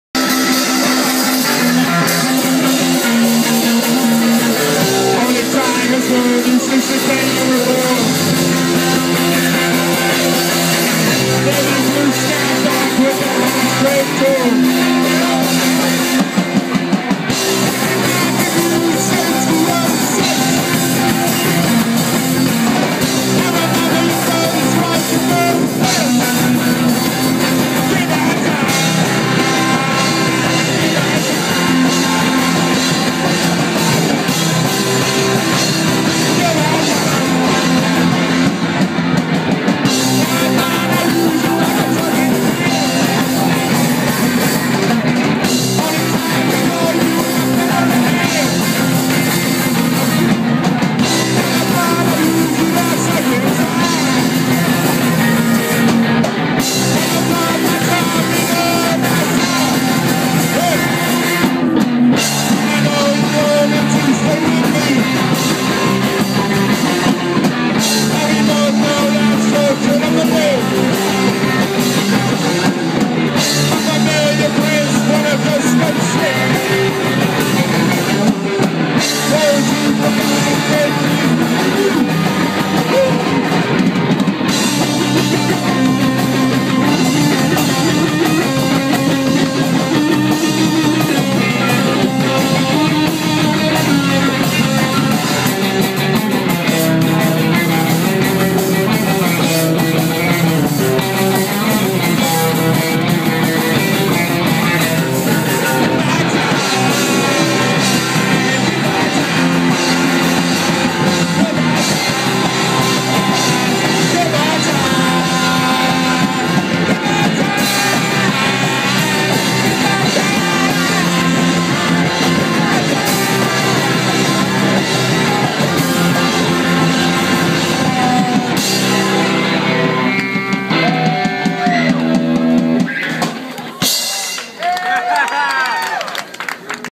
The Neighbors @ Rippers, Rockaway Beach, Queens July 27, 2013 PUNK
The Neighbors played second on this four band bill at Rippers on the boardwalk.